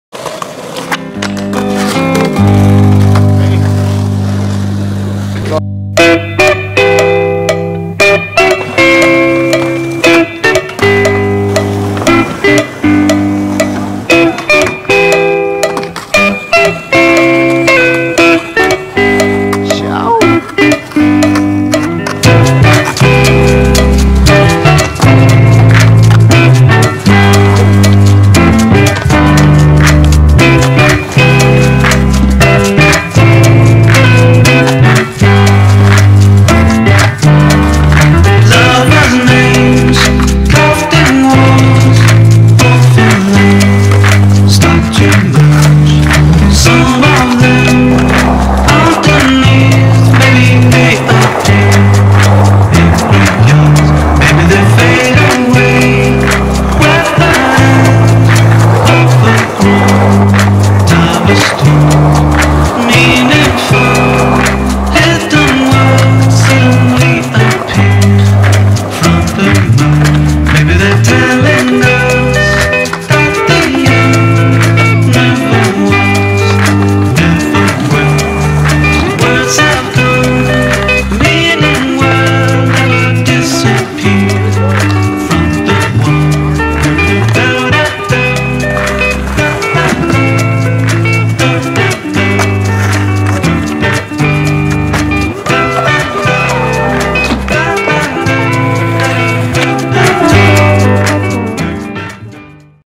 Rudy Cerda at Bobby Bonds
Me doing a couple of tricks at the skatepark.